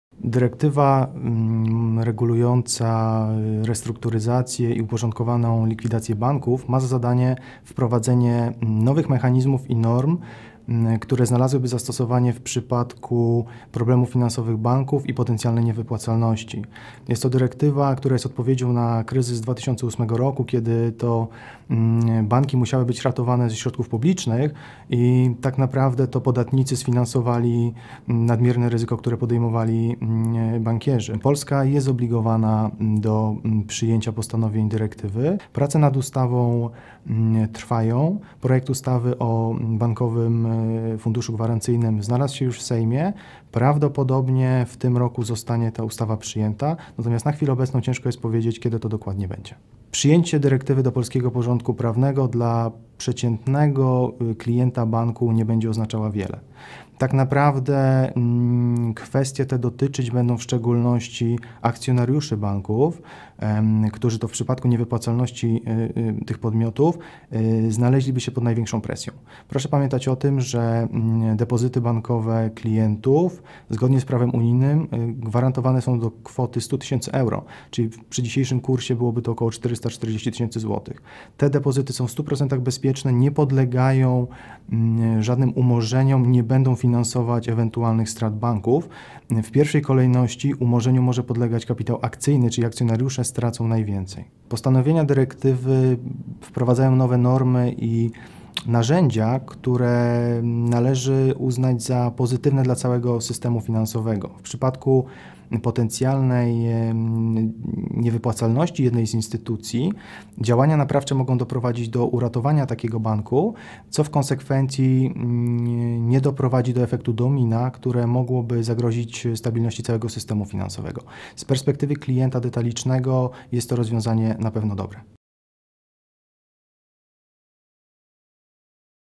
Bank będzie miał przyzwolenie na przejęcie twoich oszczędności
infoWire.pl udostępnia bezpłatnie materiały dla mediów w jakości TV. Zmontowane nagranie oraz pełna wypowiedź eksperta w formie setki do pobrania tutaj

Czy bank, który znajdzie się w trudnej sytuacji finansowej, aby uchronić się przed bankructwem, będzie mógł sięgnąć po pieniądze swoich klientów? Takie rozwiązanie umożliwia unijna dyrektywa BRRD. To odpowiedź na kryzys finansowy z 2008 r., który sprawił, że wiele banków musiało być ratowanych przed upadkiem ze środków publicznych.

Pełna wypowiedź eksperta:
– Andrzej Tomczyk, Eastern-Central Europe regional director z firmy Admiral Markets, o dyrektywie BRRD.